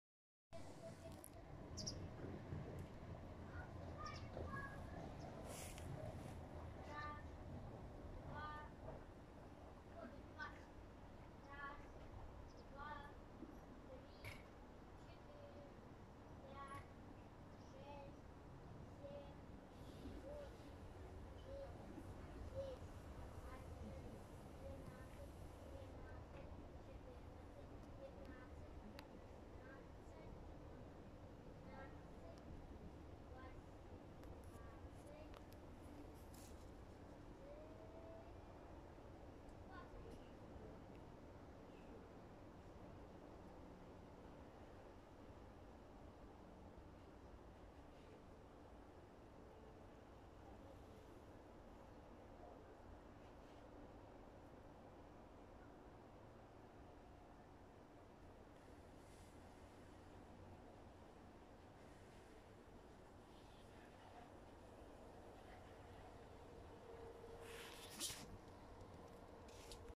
The wasp extracts nectar from the flower
A wasp that has arrived is trying to suck the nectar from the raspberry flowers.